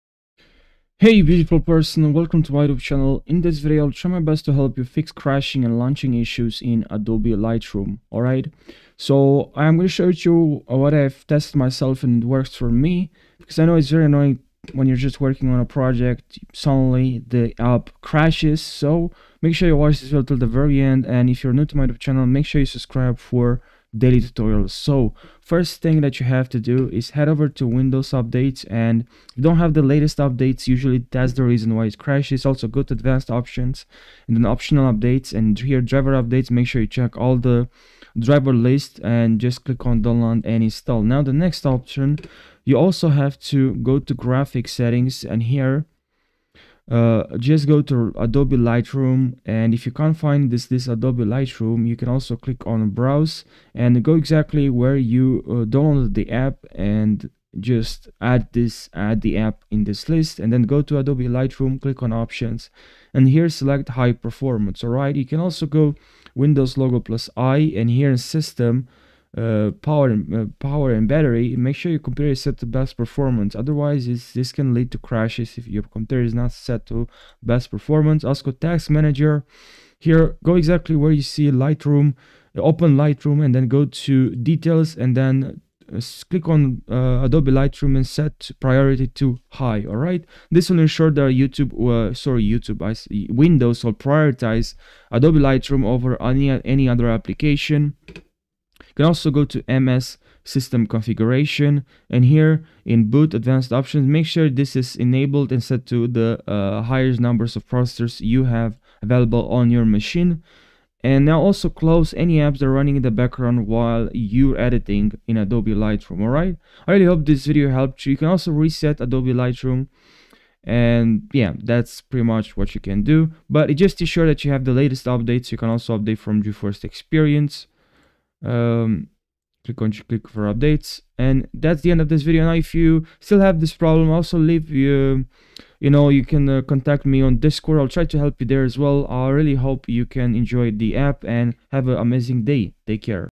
Adobe Lightroom – How To Fix Crashing And Launching Issues! Tutorial 2023
Welcome to this tutorial on fixing crashing and launching issues in Adobe Lightroom. In this video, I will guide you through the process step-by-step and help you get your Adobe Lightroom up and running again. It's frustrating when software crashes or won't launch, but by following this tutorial, you can quickly resolve any issues you may be experiencing. So, let's get started with this step-by-step guide to fixing Adobe Lightroom crashing and launching issues in 2023.